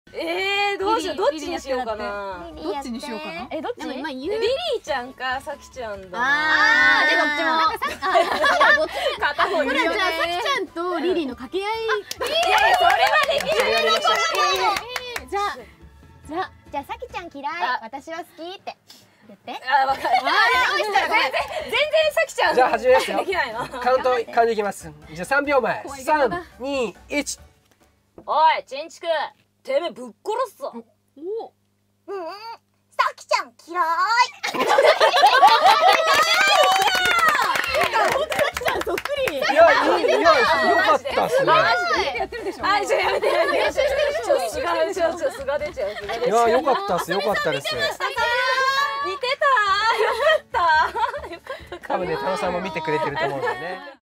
[Eng Sub] Kinuchan Impersonates Lily and Saki
Kinu-Chan impersonation is at high level lol...
When i heard it for the fist time i was like "Eh, is that really her voice?!"